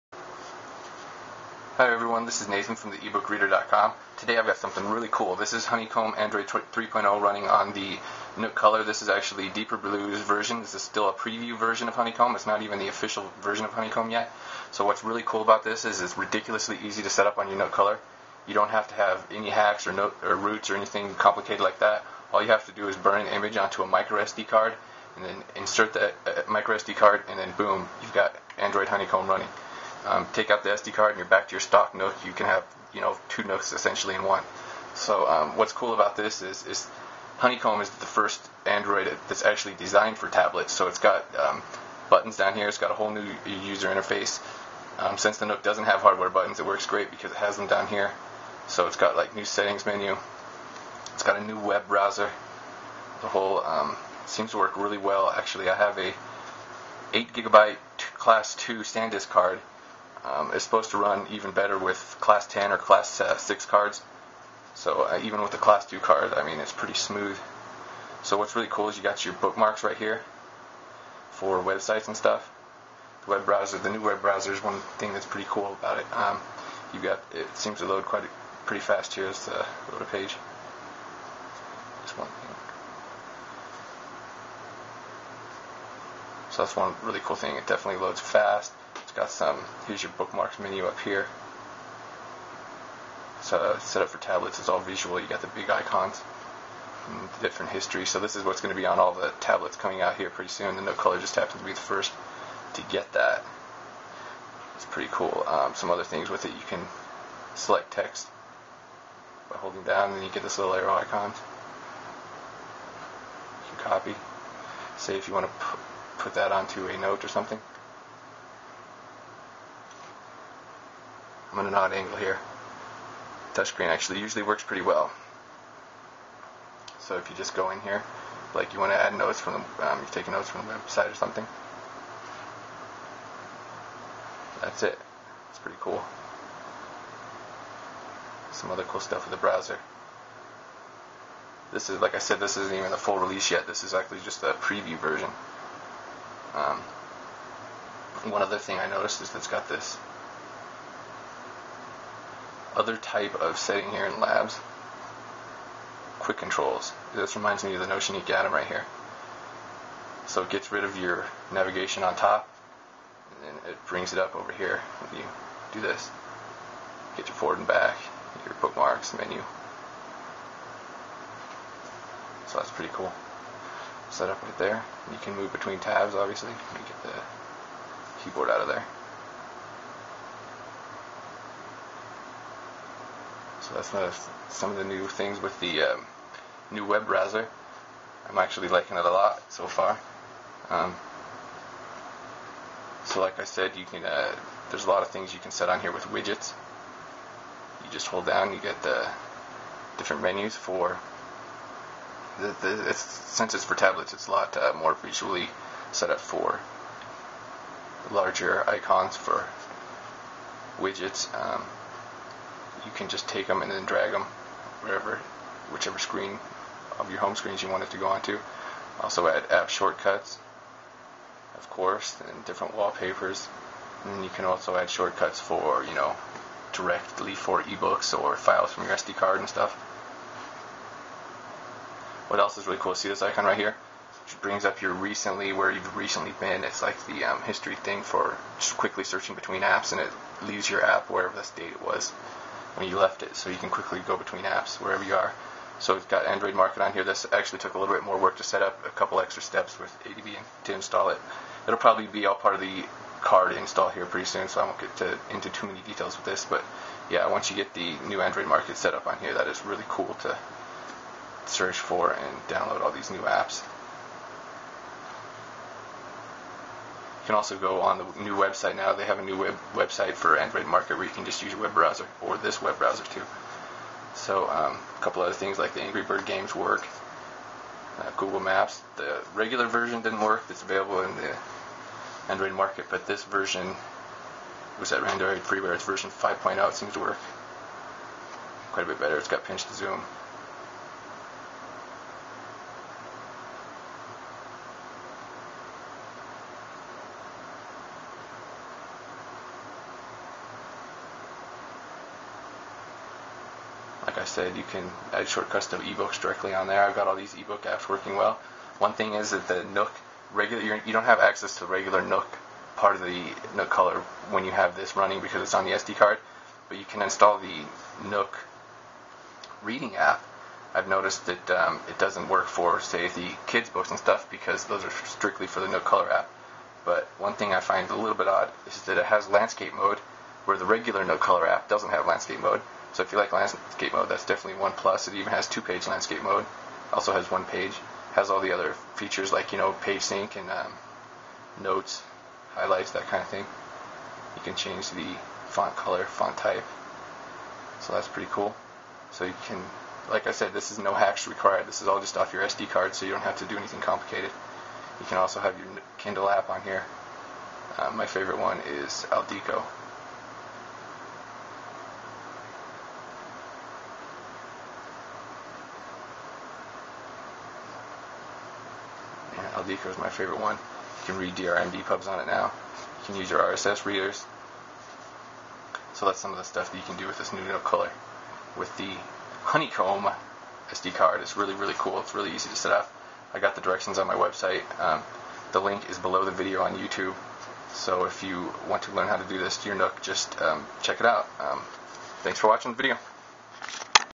Nook Color Honeycomb with Android Market Review
This video demonstrates Android 3.0 Honeycomb on the Nook Color. This is not a hack. All you have to do is save an image to a microSD card. Visit The eBook Reader.com for how to details and a ton of screenshots.